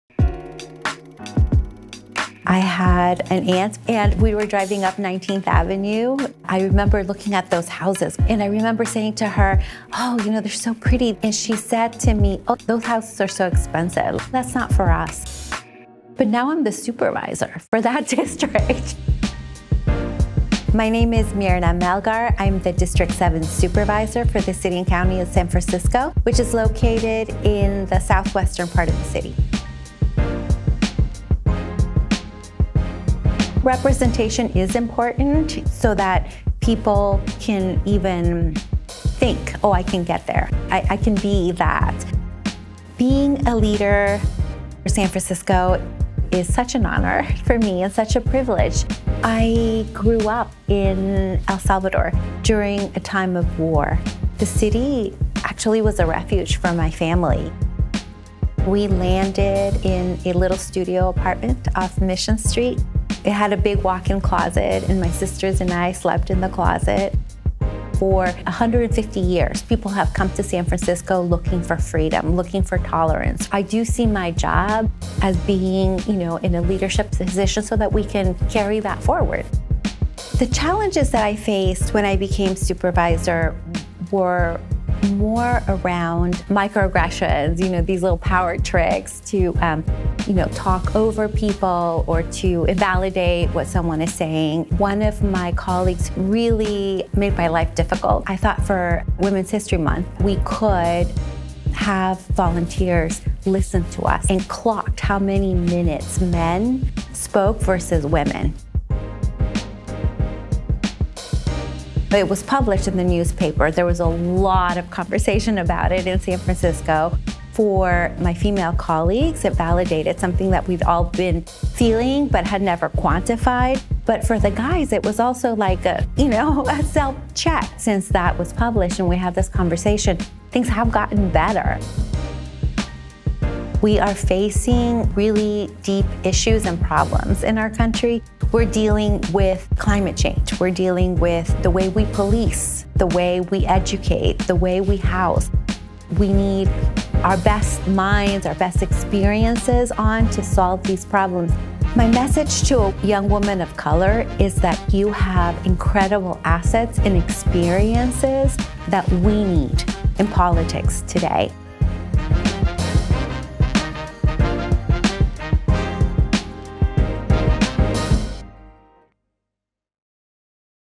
San Francisco District 7 Supervisor Myrna Melgar | Step Into Your Power
"Representation is important so that people can even think, 'Oh, I can get there.'" - San Francisco District 7 Supervisor Myrna Melgar

Supervisor Myrna Melgar has always understood the power of representation, being the first Latina elected to her role without being appointed in the Bay Area. Her experiences as an immigrant and mother have shaped how she wields her power to fight for positive change in the community.